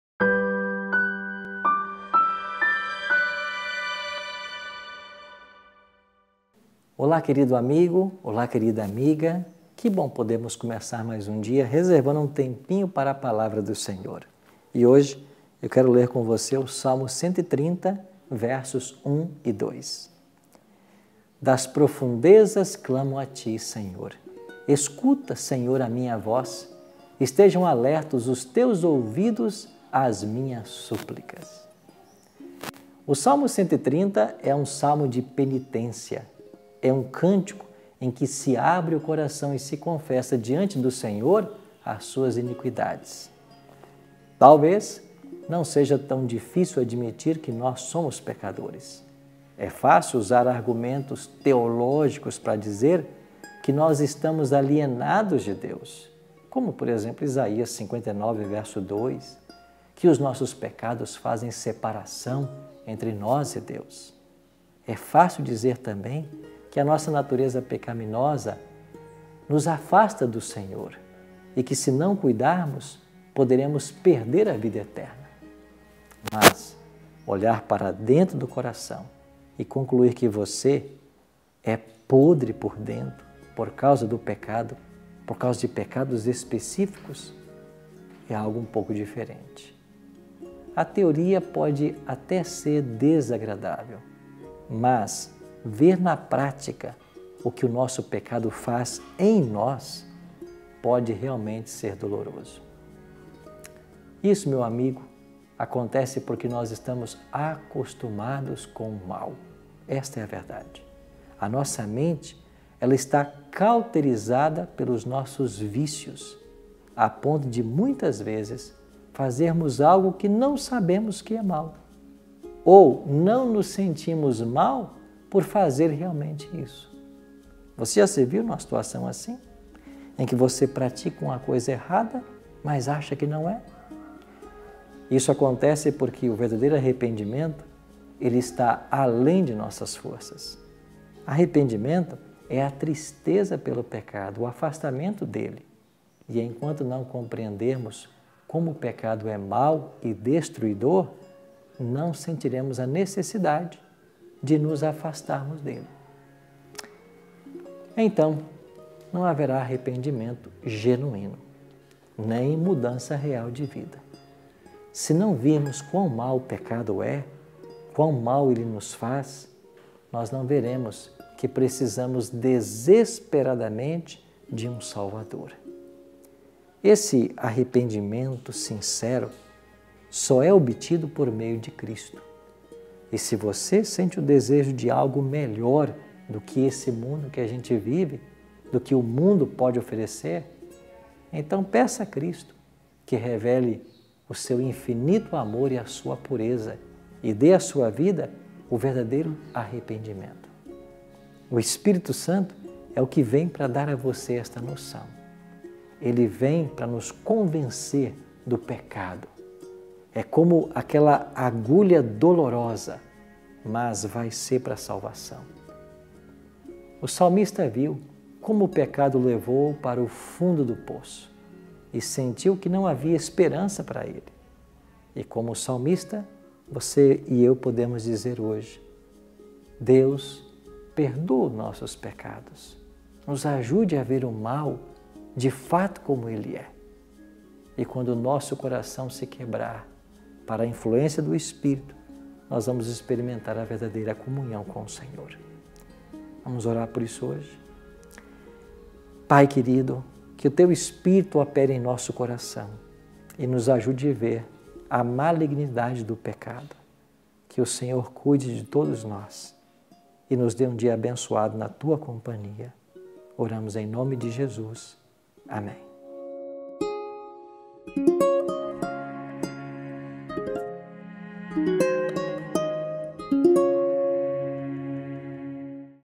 COMO ME ARREPENDER / SALMO 130 / MEDITAÇÕES DIÁRIAS / PR. ARILTON
Como podemos nos arrepender? Sabemos de coisas erradas que fazemos, mas o arrependimento é produzido apenas pelo Espírito Santo. Como Ele opera? Vamos estudar isso no video de hoje.

Este é o canal oficial do Pastor Arilton Oliveira, apresentador do Bíblia Fácil da TV Novo Tempo.